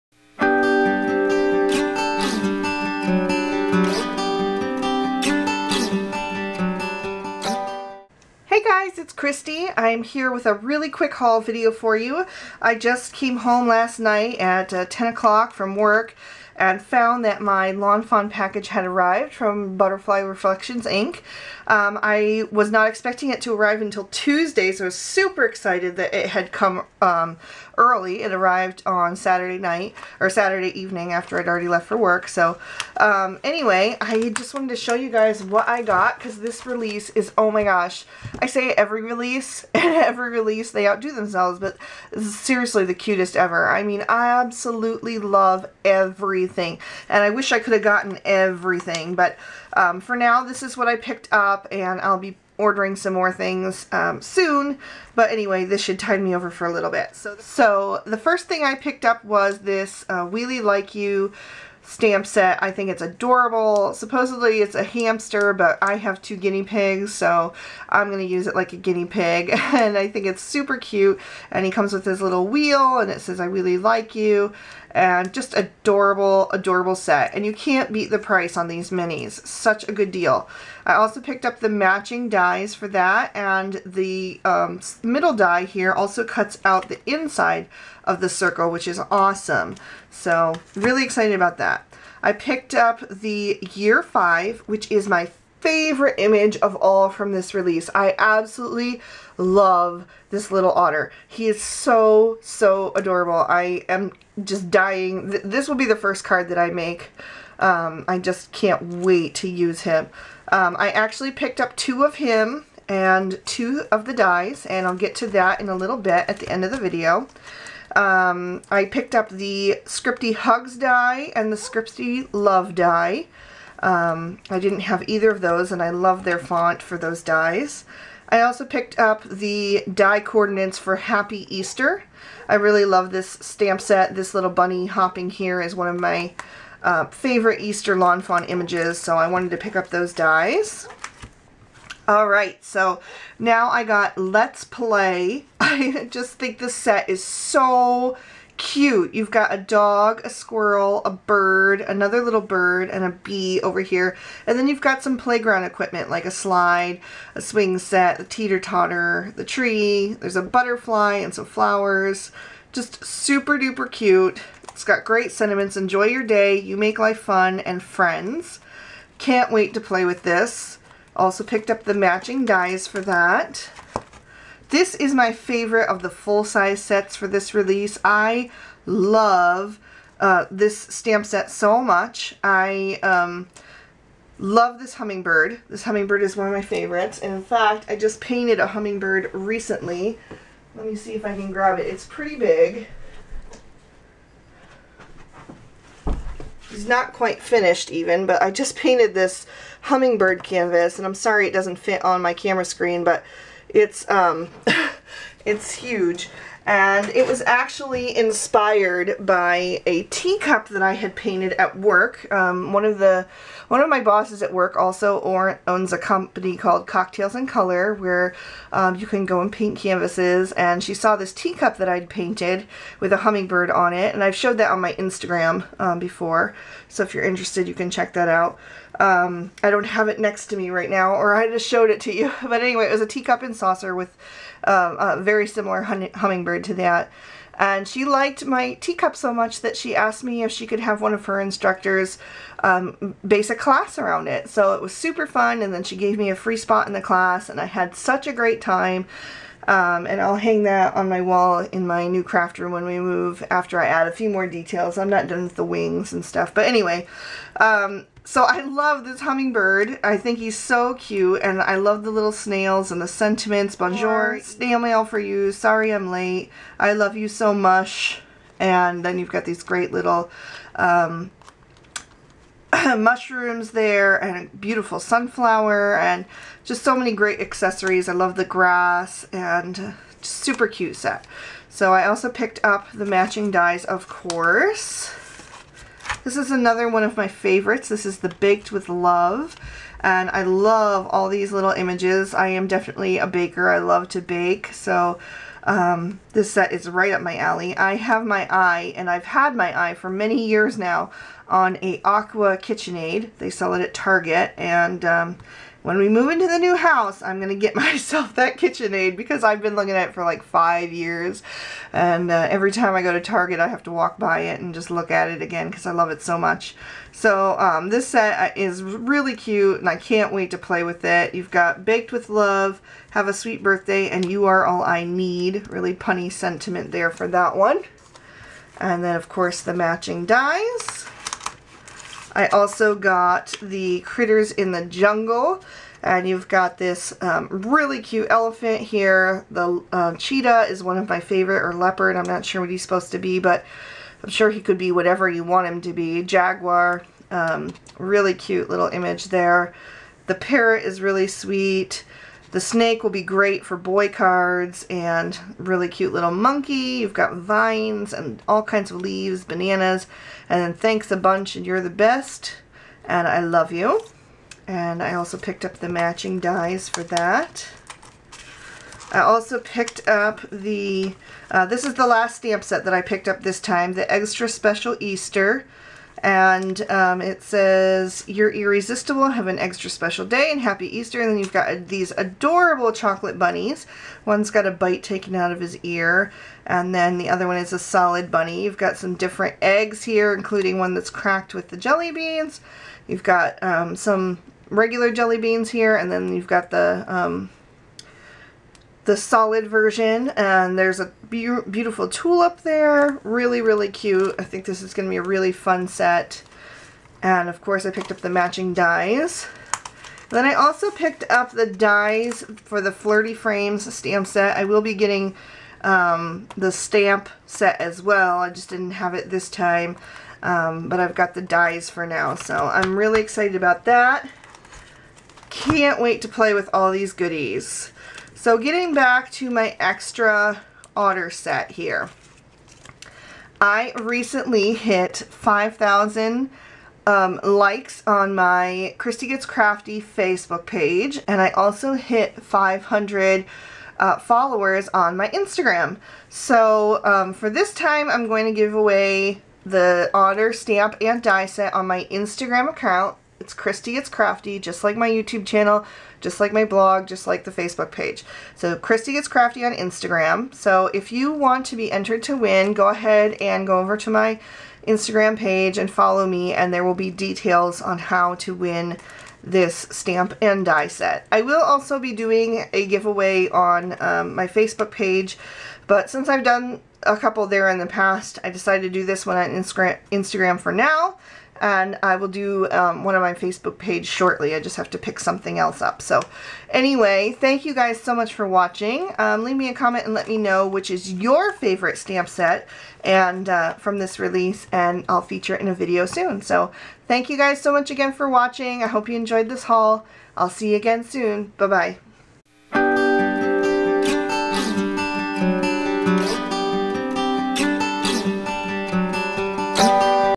Lawn Fawn New Release Haul
My latest haul of the Lawn Fawn new release from Butterfly Reflections Ink.